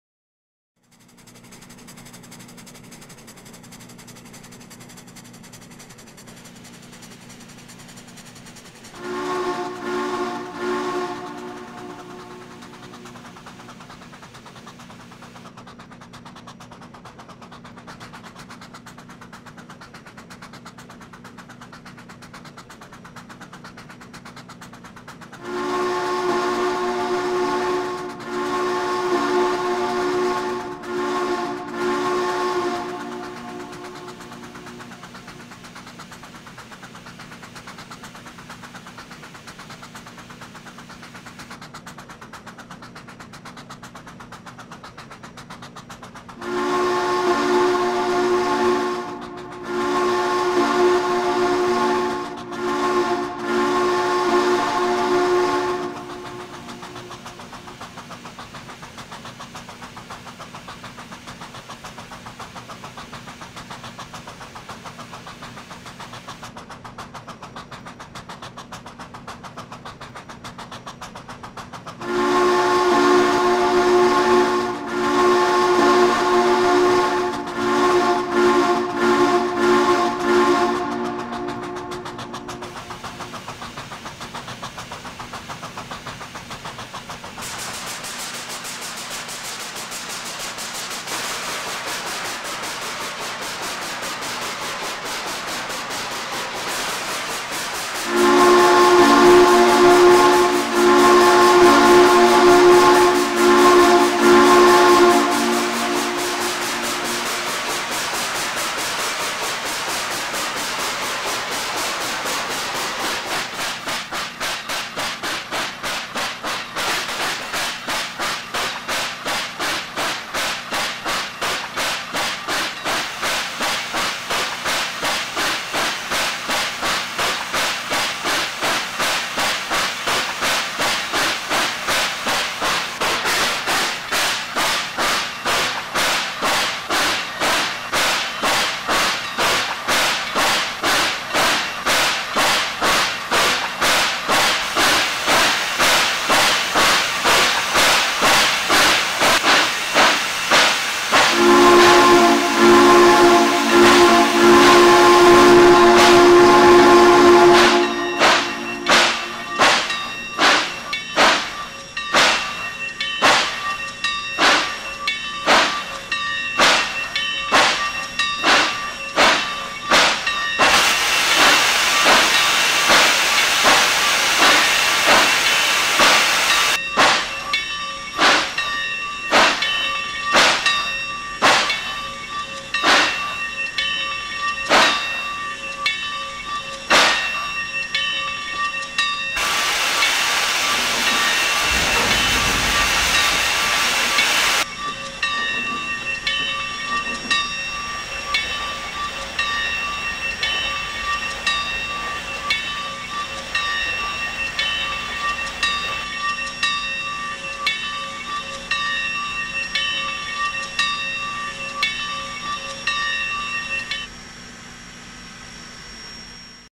Frisco 1522's Epic Adventure Day 82 Part 27
Just before making her way into the next major location on the East Coast Mainline, Frisco 1522 slows down and then stops to take on some more water at this water tower here.